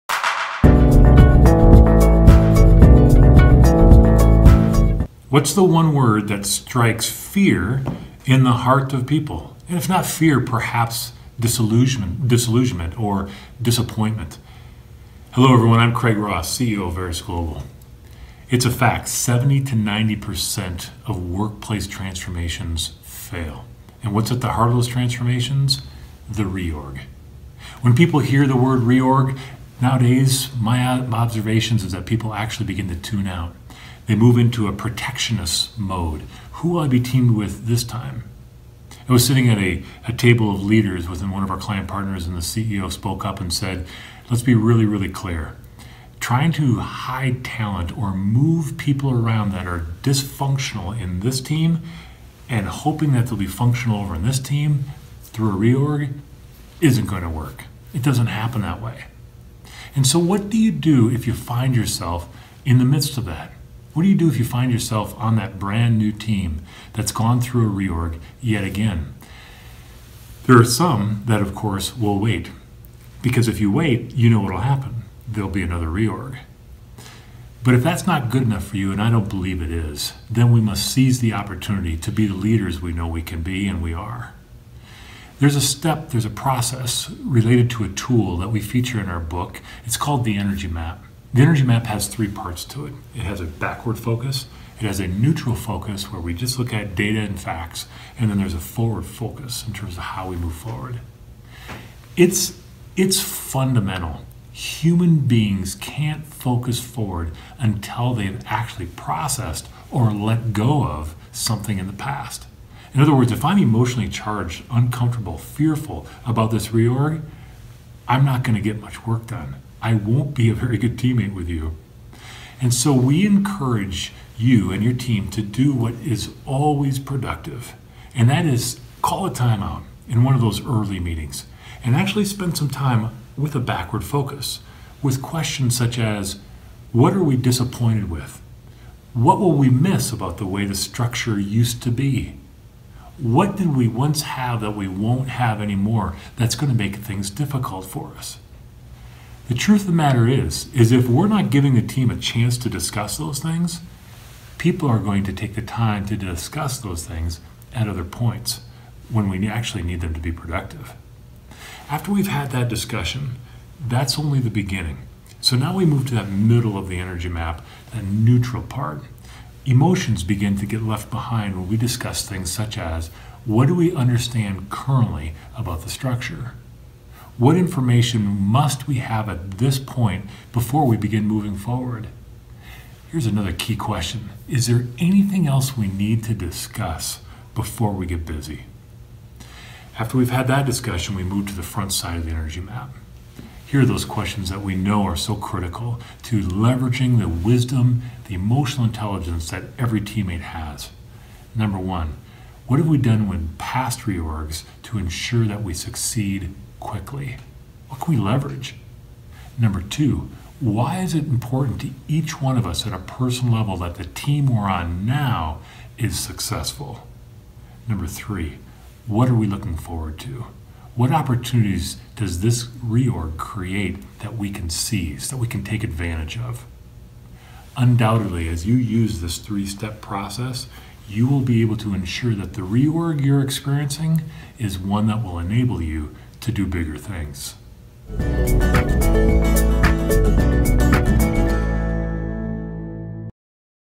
How to manage through a reorganization
70-90% of workplace transformation fail and what's at the heart of those transformation? A reorganization. What happens when you find yourself on a brand new team that has just undergone a restructure? Here's how you can seize the opportunity to be your best self.